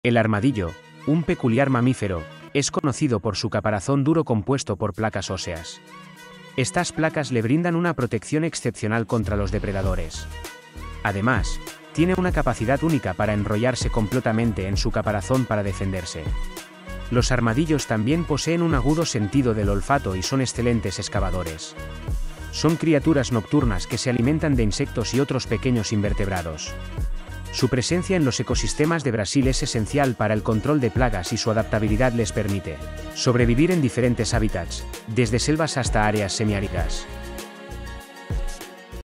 El armadillo: El guerrero acorazado de la naturaleza"
El armadillo es un mamífero peculiar que se caracteriza por su caparazón duro compuesto de placas óseas. Este caparazón le proporciona una protección excepcional contra los depredadores. Además, el armadillo tiene la capacidad única de enrollarse completamente en su caparazón para defenderse. Son excelentes excavadores y suelen alimentarse de insectos y pequeños invertebrados. Los armadillos son criaturas fascinantes y su presencia en los ecosistemas de Brasil contribuye a la diversidad y la belleza de la fauna local.
·animales